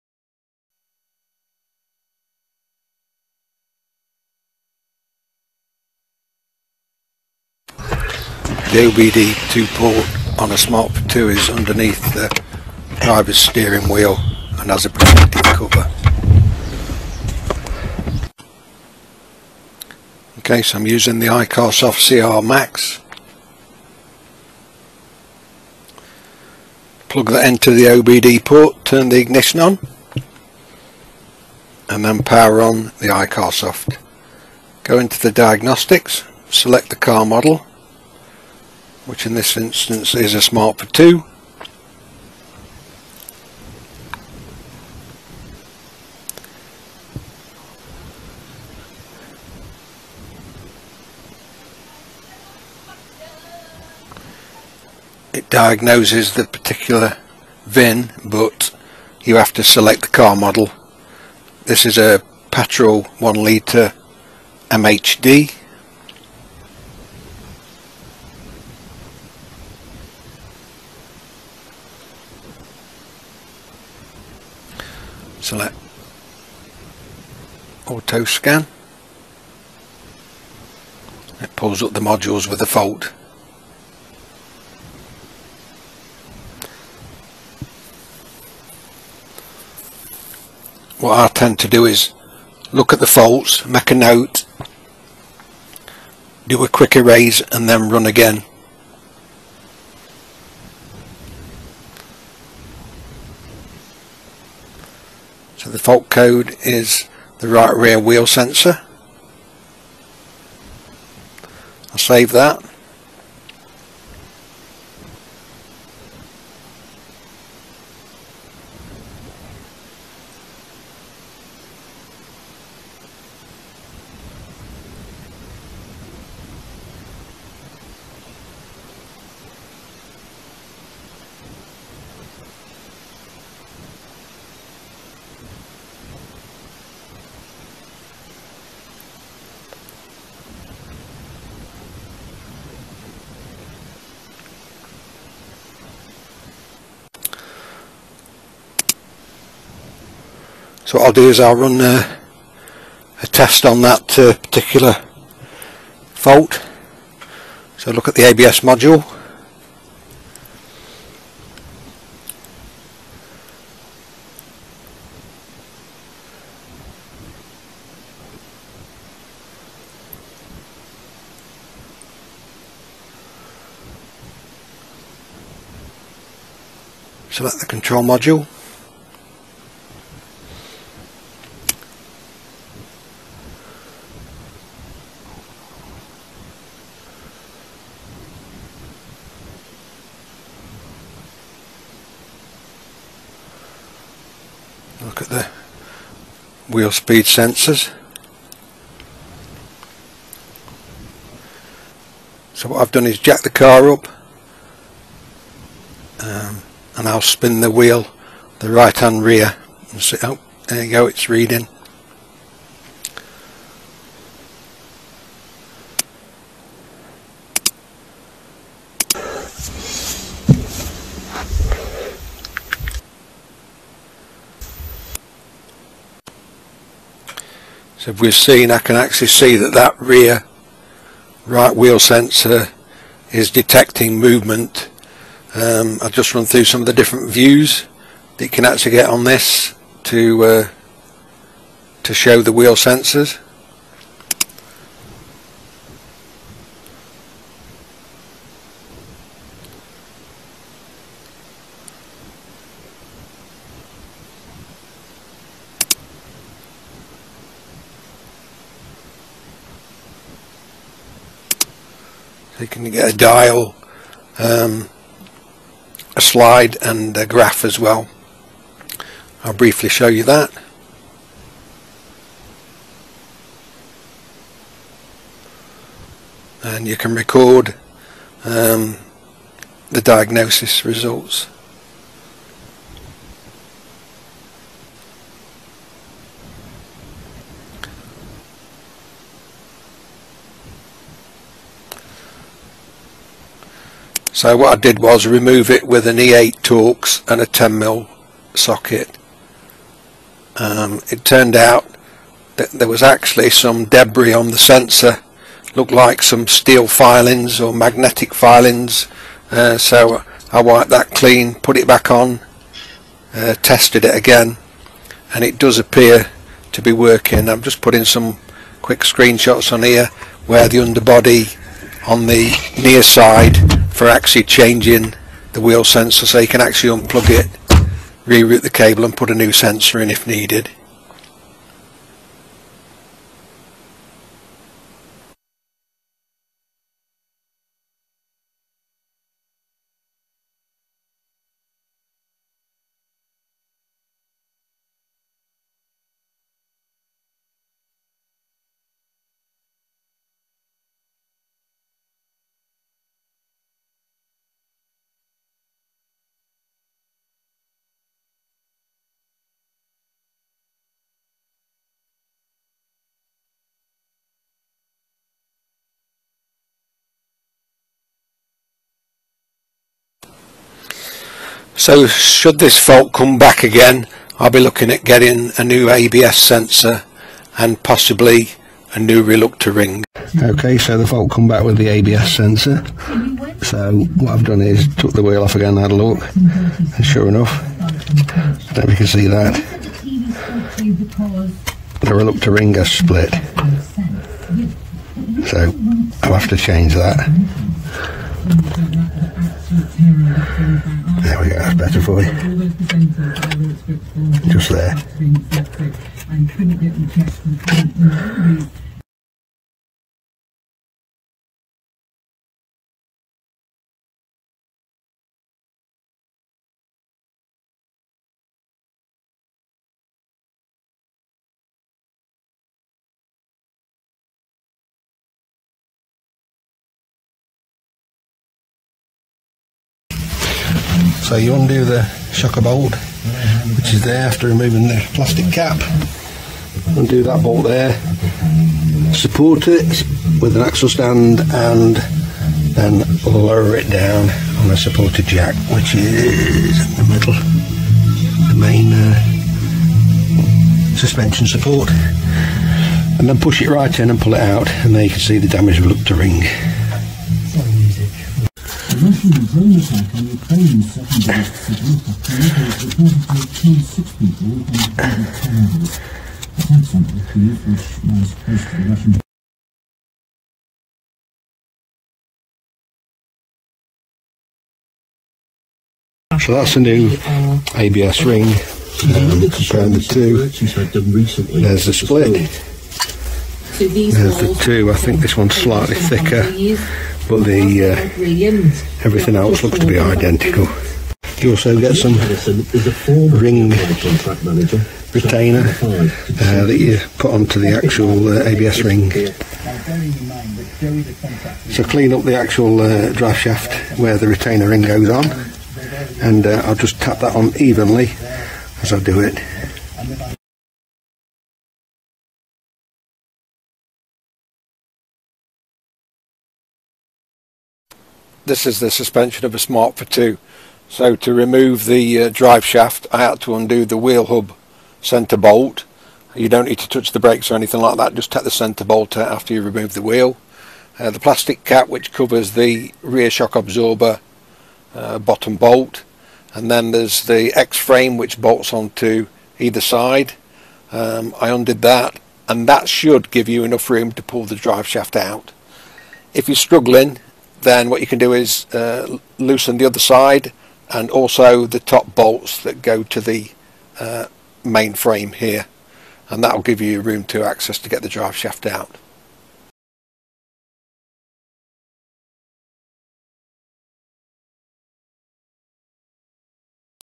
iCarsoft CR MAX - Smart ForTwo Diagnose & Repair ABS Rear sensor fault
I had an ABS error C1131 - Implausible value from right rear rpm sensor.
So i thought it would be a good time to test the CR MAX to help diagnose the cause.

As always it goes without saying. Safety first support with axle stands and jack.

additional tools needed to remove Wheel sensor
E8 torx socket
10mm spanner

I cleaned the wheel sensor as it had some magnet steel filings on it
hopefully it wont re appear.

otherwise i'll have to look at ABS sensor and or Reluctor ring.

In conclusion the iCarsoft is more than adequate for the ABS sensor diagnosis

So the fault reappeared and as seen in the video the reluctor ring was split. A new reluctor ring was purchased. Driveshaft removed from one side which may be easier to remove completely.

Jack and axle stands to support car,
Remove wheel.
Undo driveshaft bolt
Remove shocker plastic cover from bottom bolt.
Remove shocker bolt
This may be enough to remove the Driveshaft when lowering the horse shoe shape lower suspension frame.
If you decide to remove the driveshaft(tap Driveshaft out with Drift and hammer) you can use a chisel with car between gearbox and driveshaft in the correct place with thicker raise casting on gearbox, remember to  capture gearbox oil and reuse.
I found the driveshaft awkward but managed on the car. Loosening the X frame bolts to allow movement.
Prepare and fit new reluctor ring - You may find it easier to preheat reluctor ring in the oven, I ended up using a grinder flap sand disk and using the old reluctor ring to tap the new one on so as not to damage the New reluctor ring.

Hope you found the Video useful

A great source for info on all Smarts is evilution